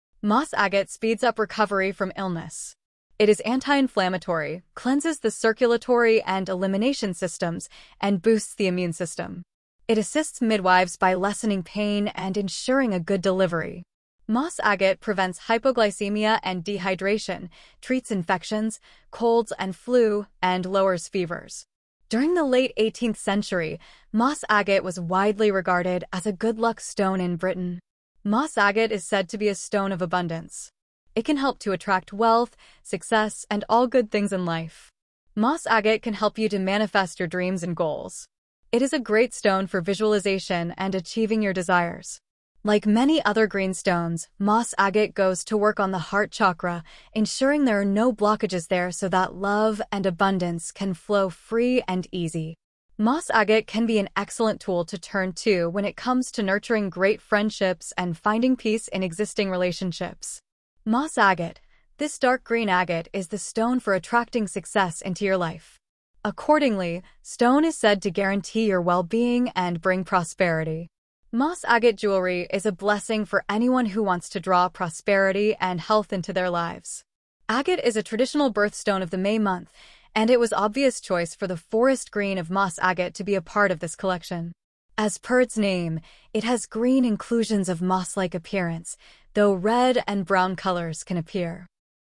Let us know the benefits of MOSS AGATE! Let us know when and where to wear AGATE stone? {Episode 29}
Our Hastags :-